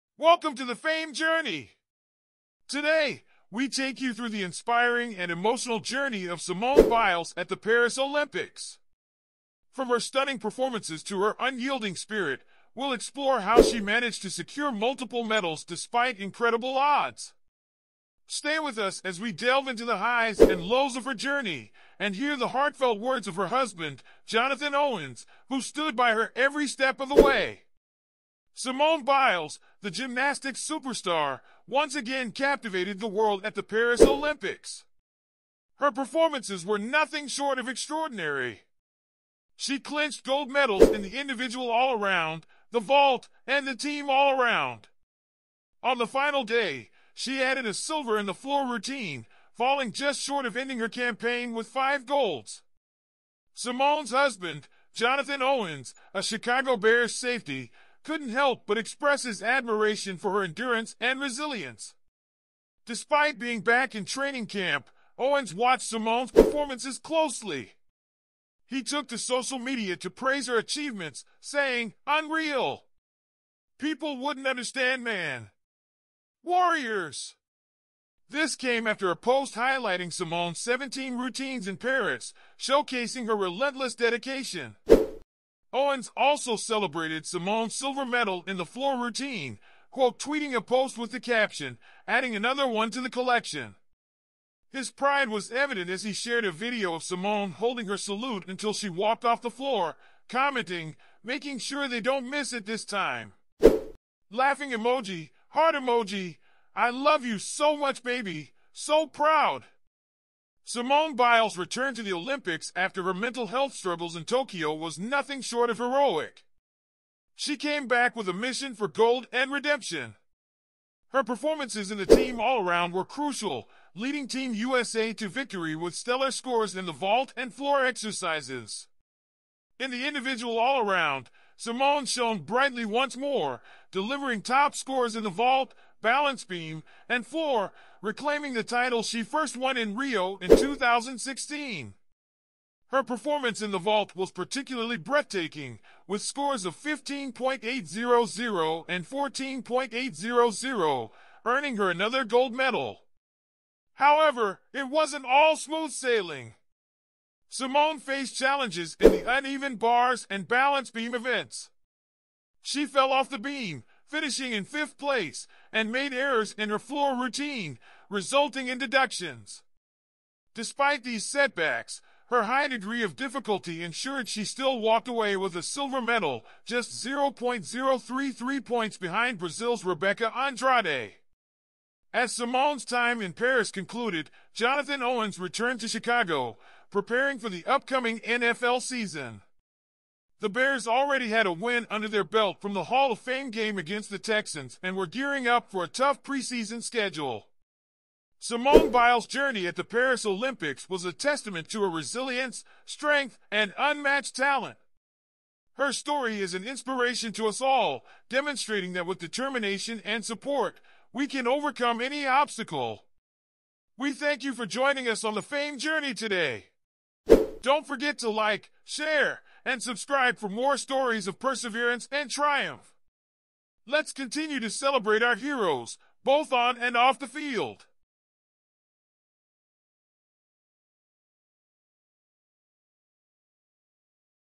Biles' Fiancé Owens: Fans 'Can't Understand' Her Endurance After Olympic Shortfall
Welcome to The Fame Journey. Today, we take you through the inspiring and emotional journey of Simone Biles at the Paris Olympics. From her stunning performances to her unyielding spirit, we’ll explore how she managed to secure multiple medals despite incredible odds. Stay with us as we delve into the highs and lows of her journey, and hear the heartfelt words of her husband, Jonathan Owens, who stood by her every step of the way.